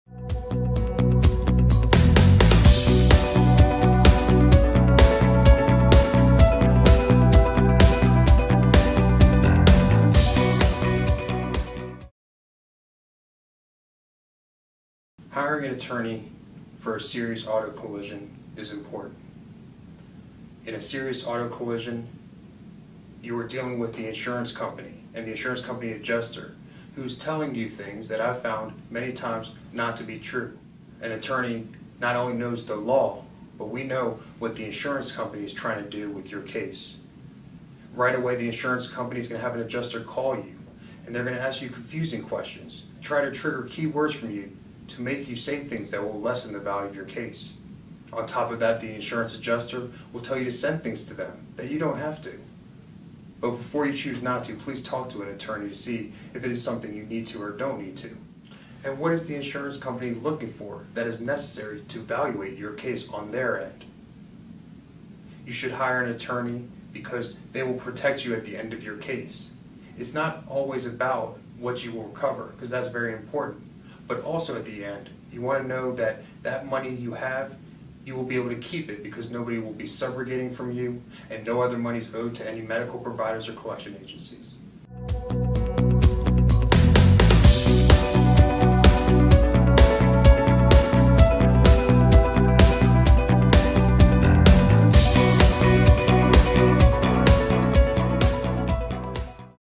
Michael S. Wilensky - Is hiring an attorney important for a serious auto collision?
Michael S. Wilensky, one of Georgia’s premier wrongful death and serious injury attorneys, explains that an attorney not only knows the laws, but we know what the insurance company is trying to do with your case.
Michael S. Wilensky, LLC
One Atlanta Plaza
950 East Paces Ferry Rd. NE, Suite1625
Atlanta, Georgia 30326